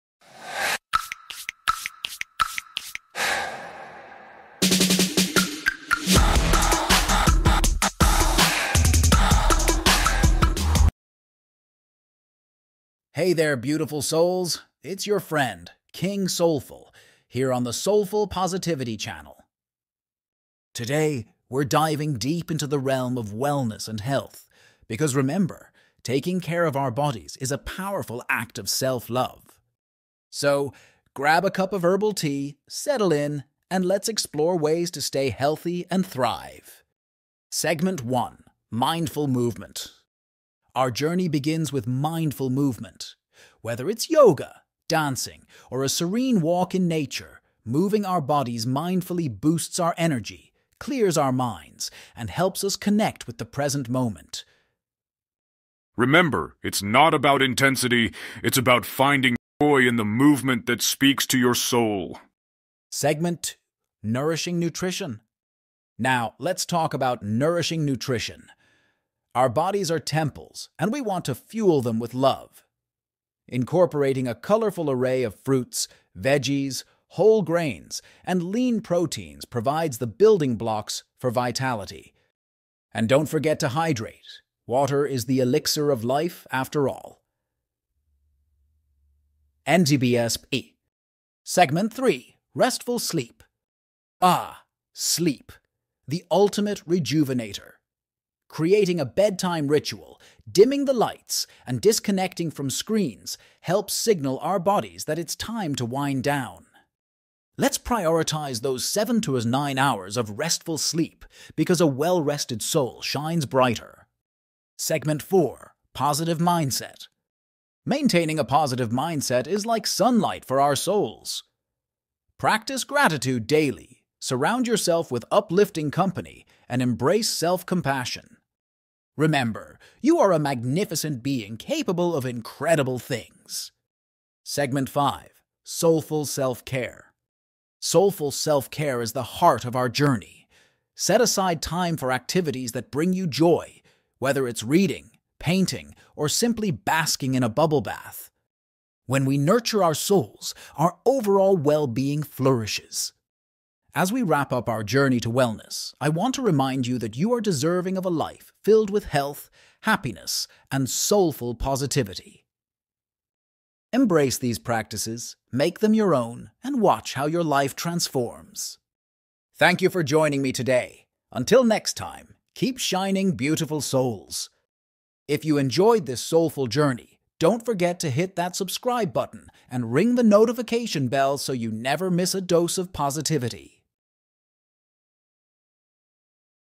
Starting a healthy lifestyle
Welcome to the Soulful Positivity YouTube channel! 🌱 Join us on a transformative journey towards a healthier and more vibrant life with our new series, "Starting a Healthy Lifestyle." In collaboration with King Soulful, this series is your ultimate guide to embracing wellness, nourishing your body, and cultivating a positive mindset. Get ready for expert tips, motivational insights, and practical advice that will empower you to make lasting changes. Let's thrive together! 💪🌟